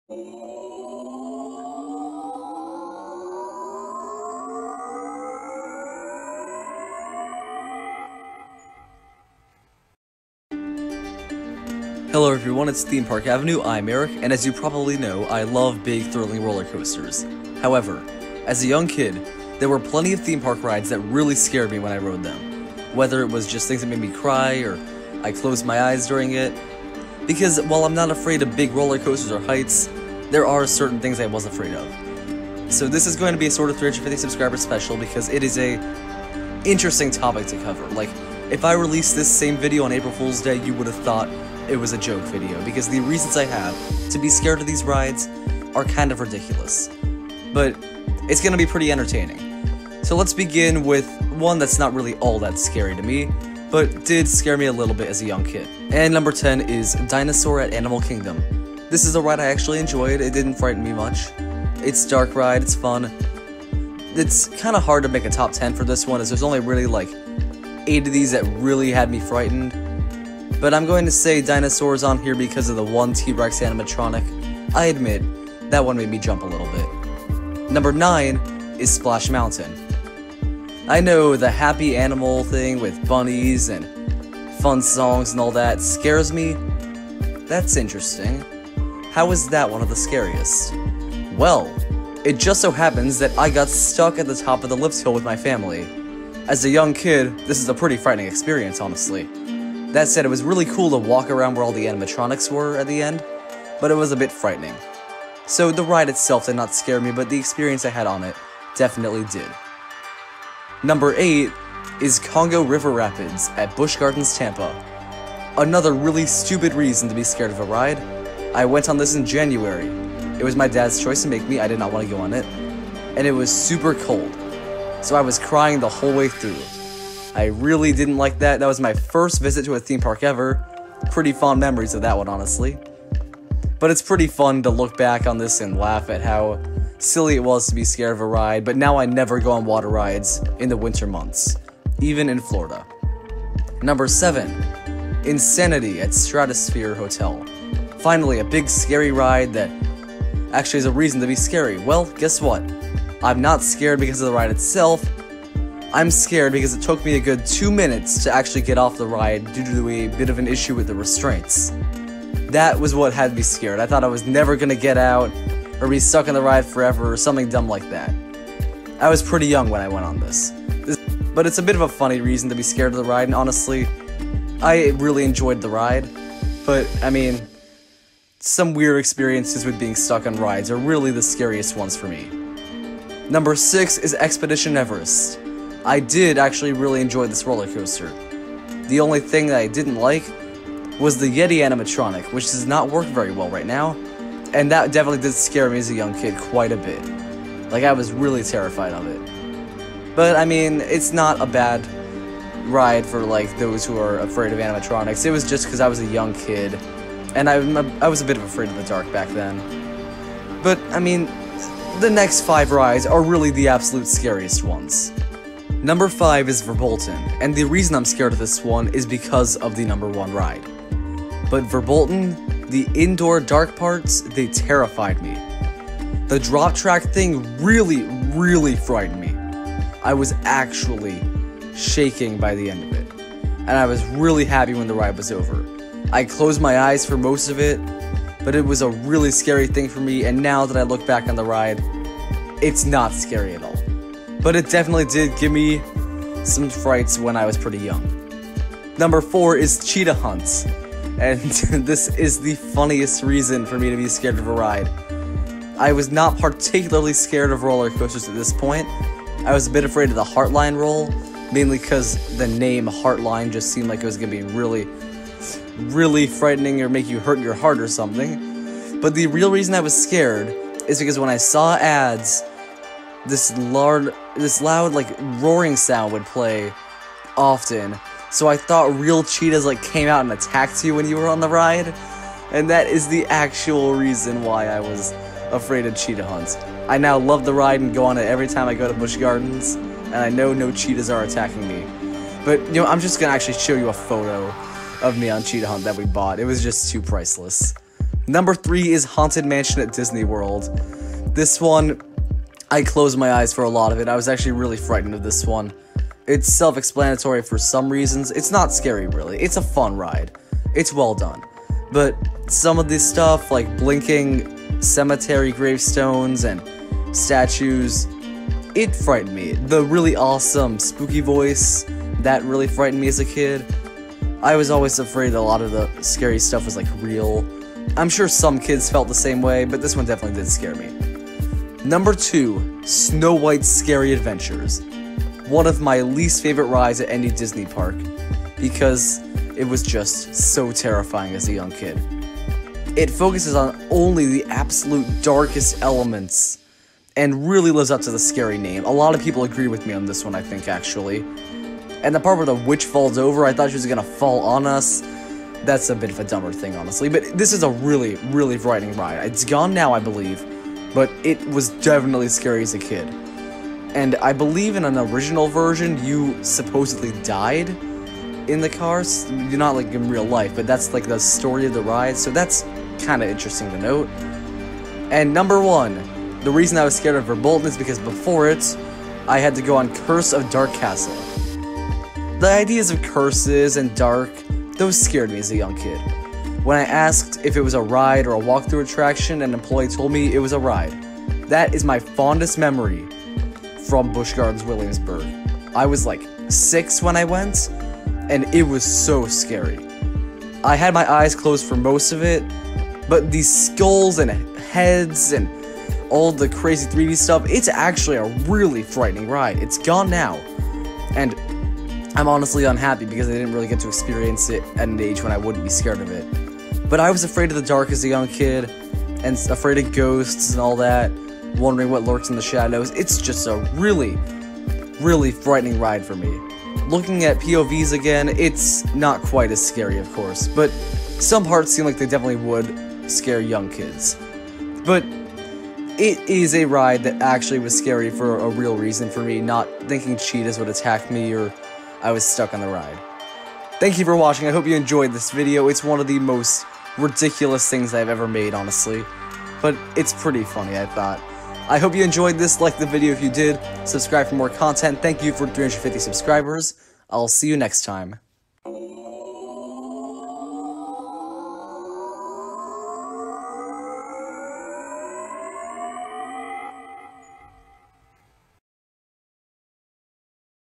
Top 10 Theme Park Rides That Scared Me As a Kid (350 Subscriber Special)
I love the tallest, fastest, and scariest roller coasters and thrill rides. However, I used to be frightened of many dark rides and some roller coasters as a kid. In this video, I will be discussing the scariest theme park rides and roller coasters for young me.

Credit to AmusementInsider for some of the footage.